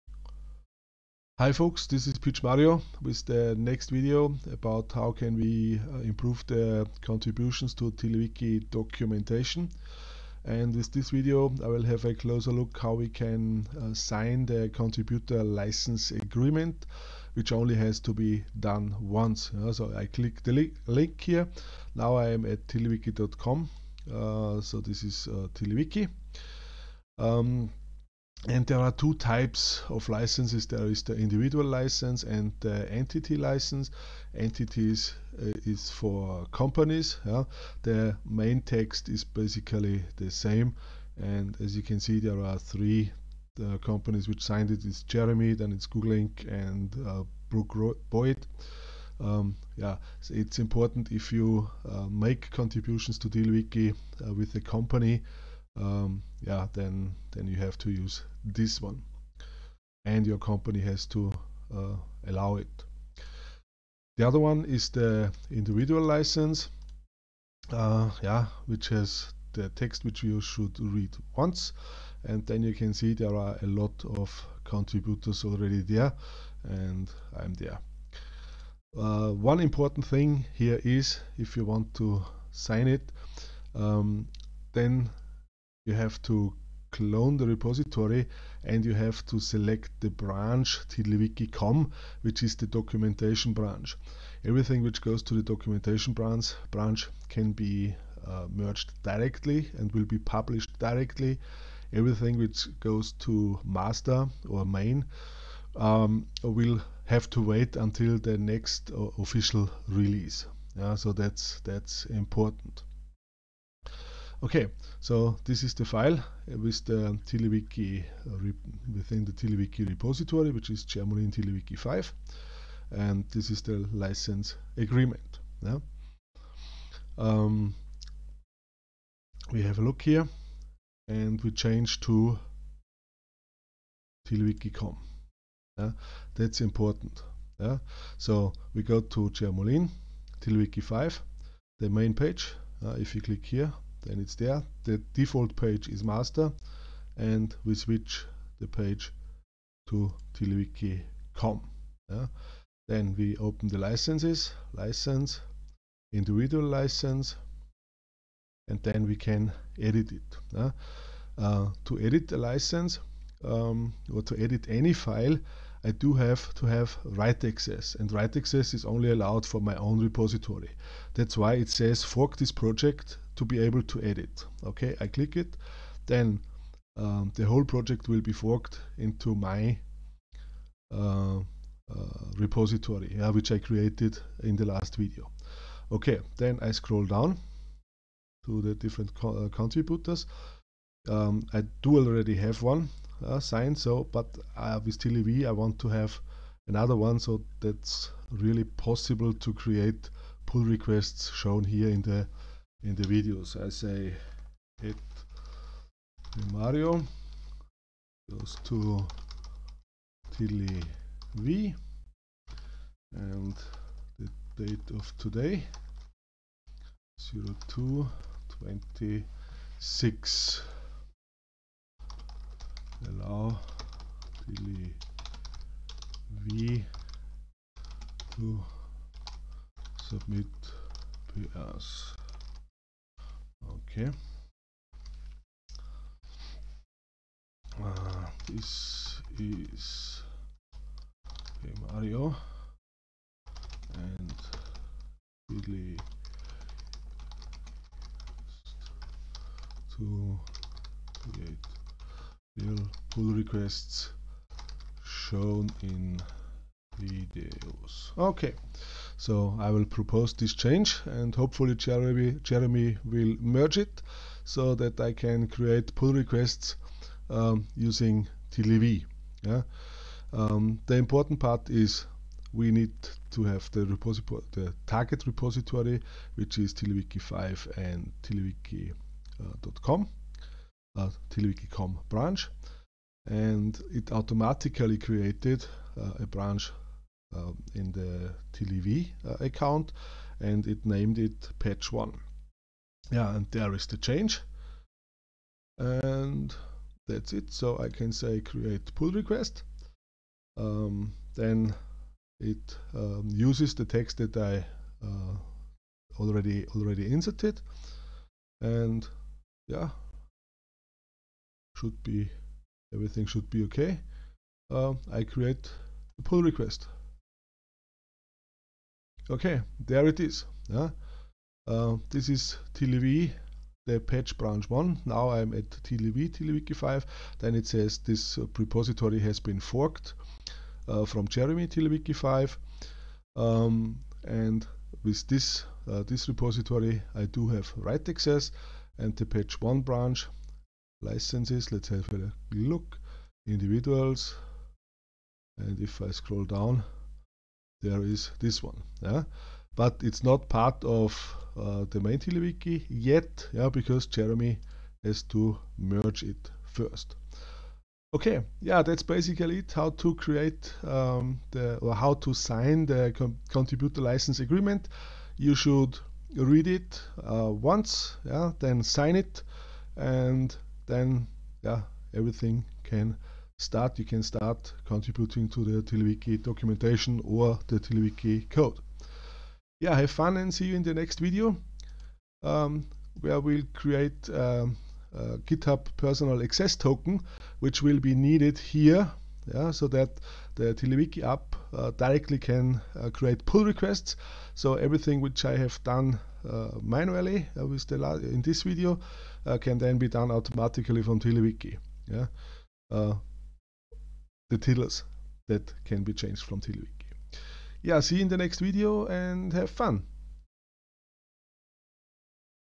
02 Sign TW CLA using GitHub Web UI
A short video, how to sign the TiddlyWiki Contributor License Agreement using the GitHub Web UI